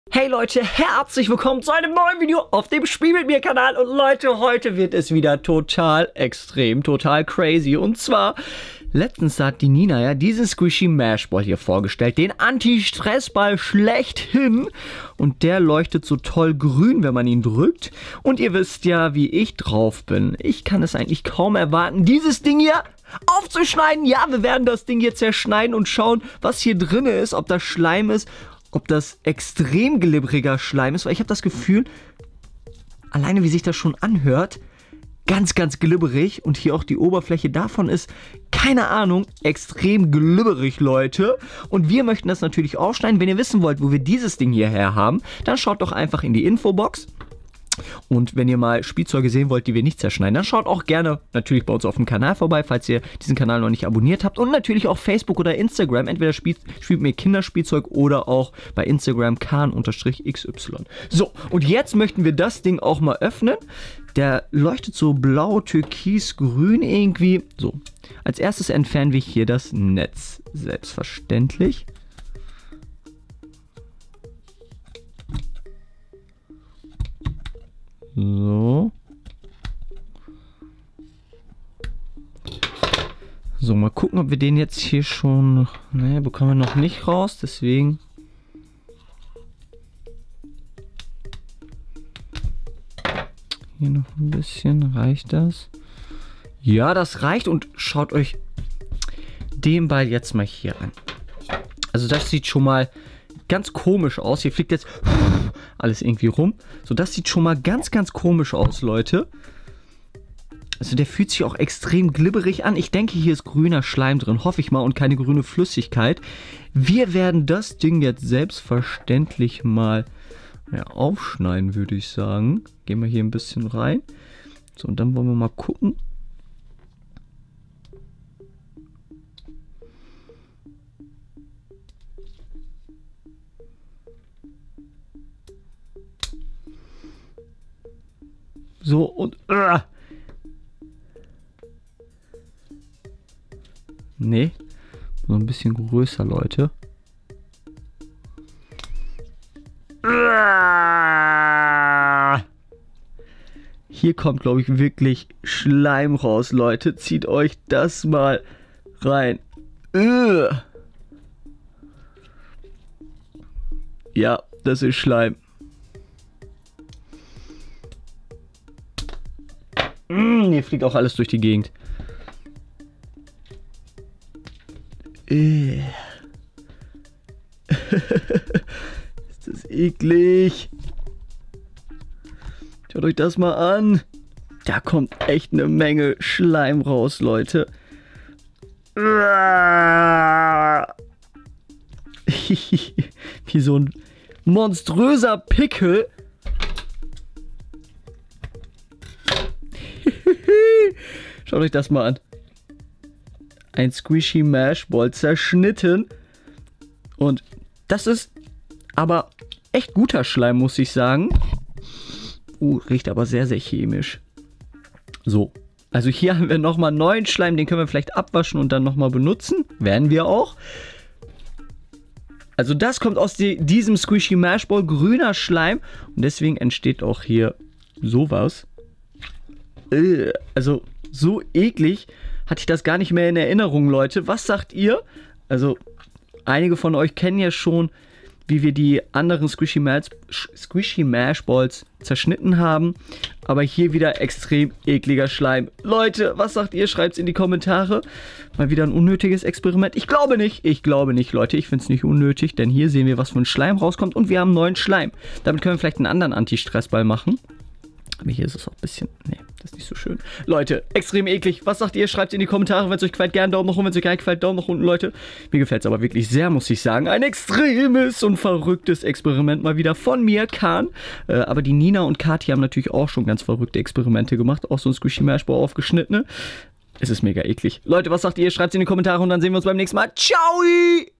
Squishy Mesh Ball zerschneiden (mal wieder)! Ist das ekliger Glibber Schleim? Pickel ausdrücken?!
6395